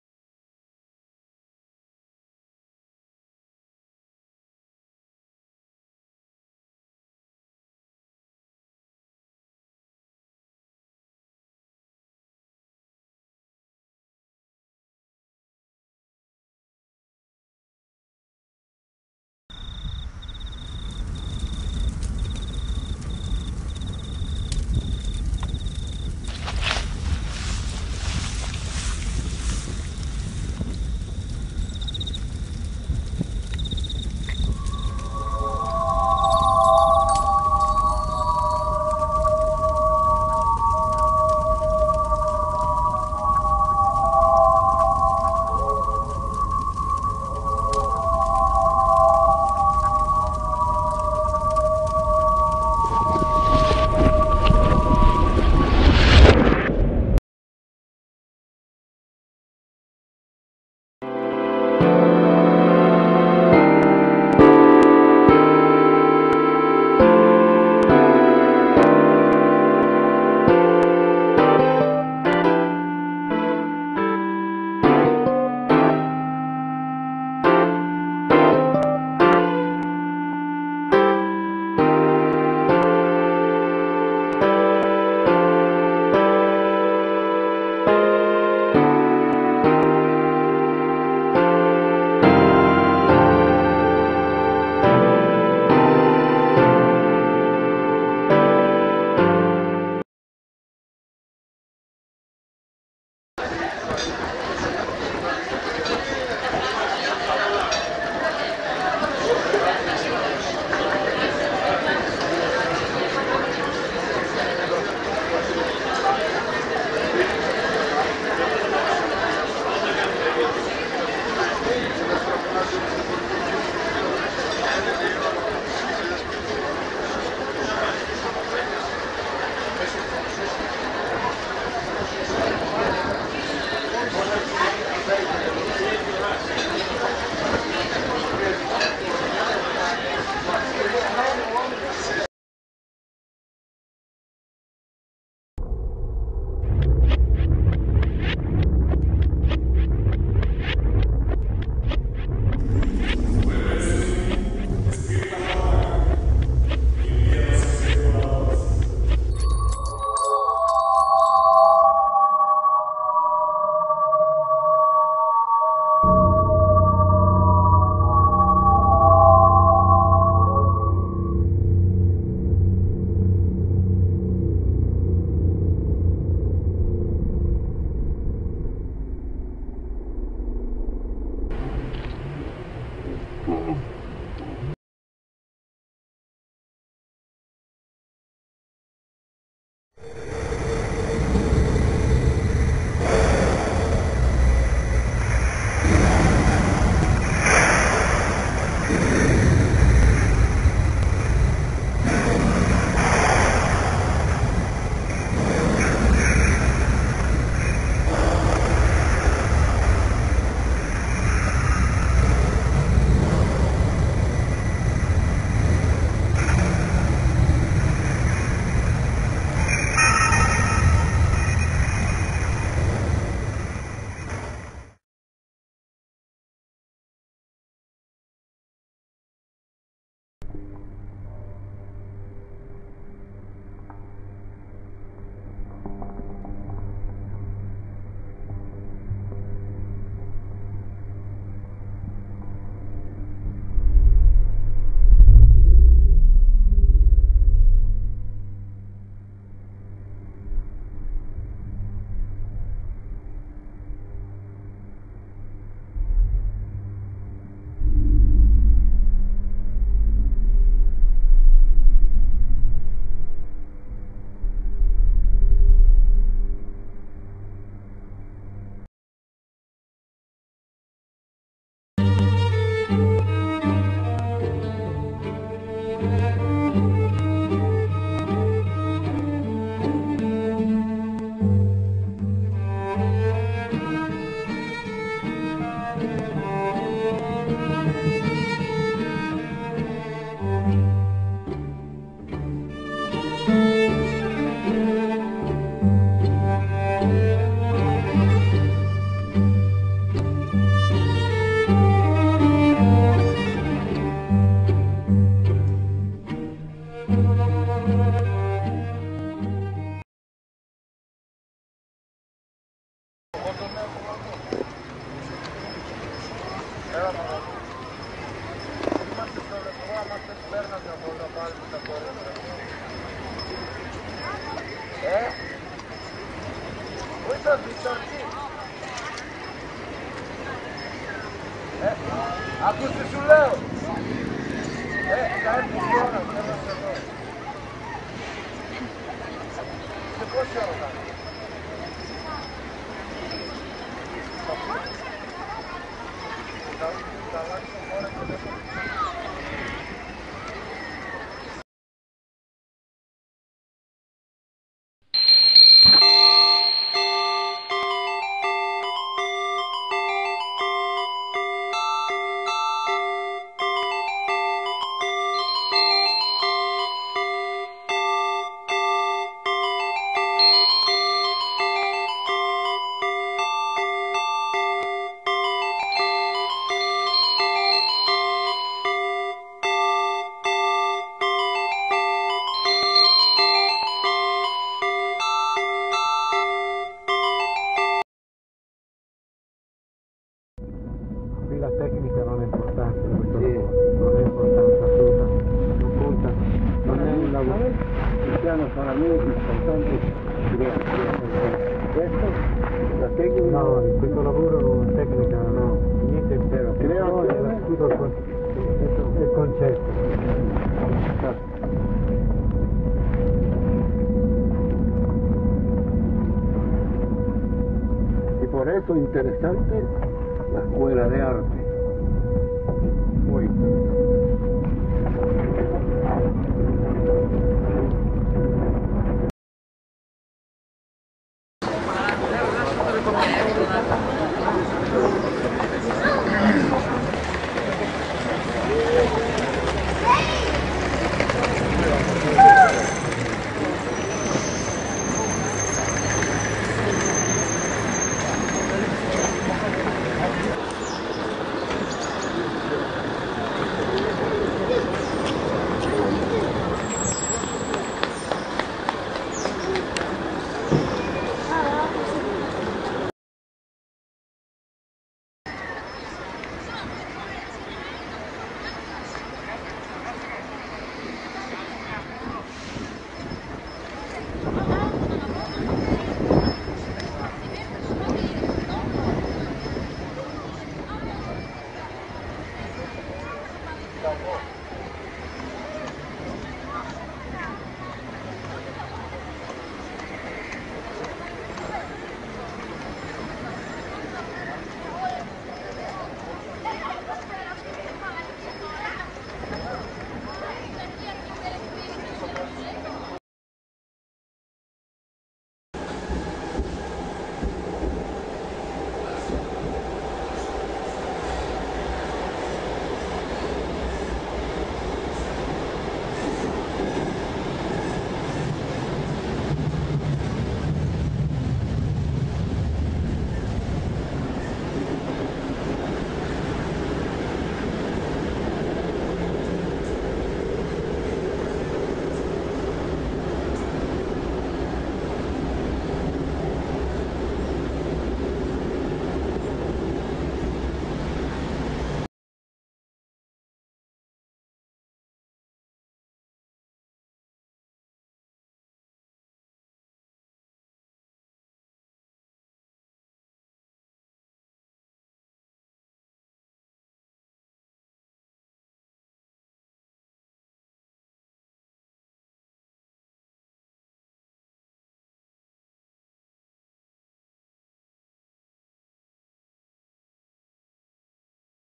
segni 20+20 - VUT 2009
SEGNI 20+20= V.U.T. - Video Underground Torino Rassegna di video d'Arte in 40" - Gli Artisti: Luciano Massari, Cosimo Terlizzi, Christian Rainer, Antinea Peruch, Andreina Polo, Raffaele De Martino-ZUD, Paola Zorzi, Rassegna a cura di Delia Gianti e Max Ponte con il contributo di Mariangela De Piano - Trasmesso sugli schermi della Metropolitana di Torino a novembre del 2009 - in collaborazione con GTT e ATTS - con il contributo della Regione Piemonte